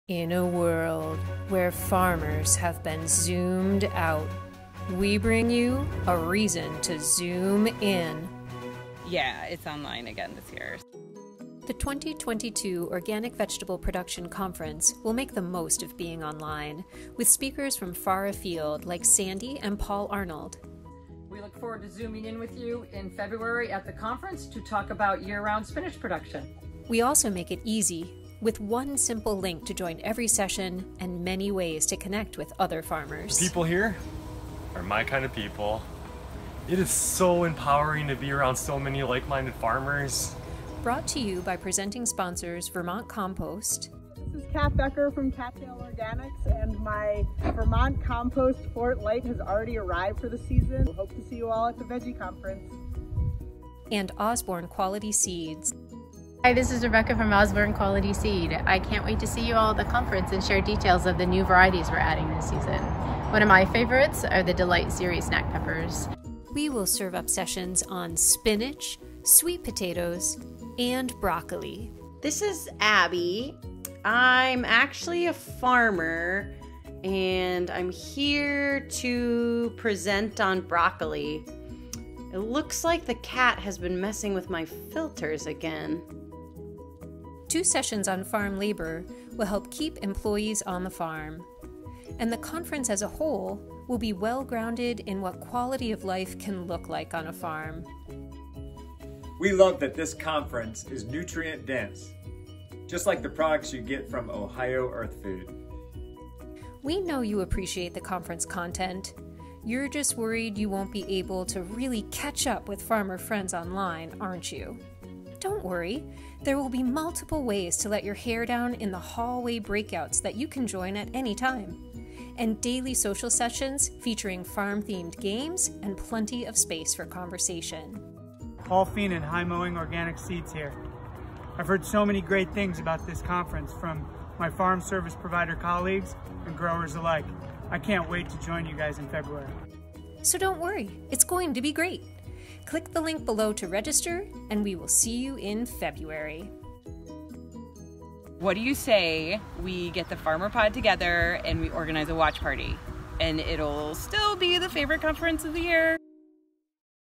2022 Organic Vegetable Production Conference Trailer
Join us in February for the 2022 Organic Vegetable Production Conference.  Learn more and register here: